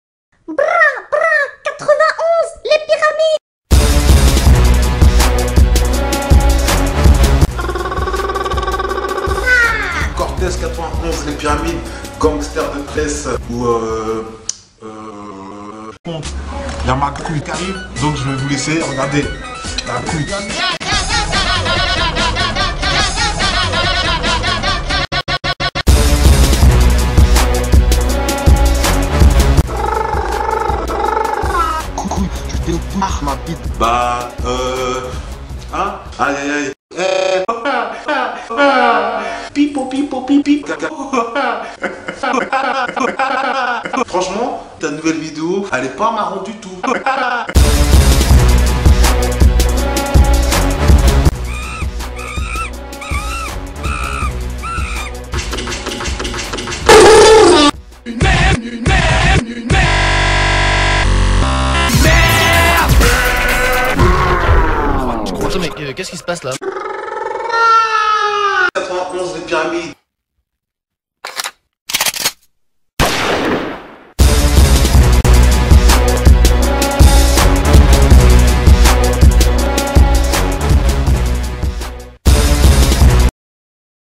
Cortex, Gangster 2 Fesses
En hommage à tous les poopeurs qui détournent Cortex (Jefaischierlesgens, Cauetinfo2, 123Lunatic, Alexthekiller10...) et à Cyprien, j'ai fait un petit best-of des détournements de Cortex sur Youtube. Du coup, cette vidéo servira aussi à ma nouvelle série-poop que je prépare sur le Gangster 2 Fesses des pires ramides dans son cul (après une série sur Morsay, cela va de soi).
Au passage j'emmerde ces deux grosses sous-merdes infinies du web bien profond et je tiens à rappeler que s'ils ne cessent de publier des vidéos qui n'ont pas lieu d'exister, cela peut au moins servir de source pour plein de Youtube Poops !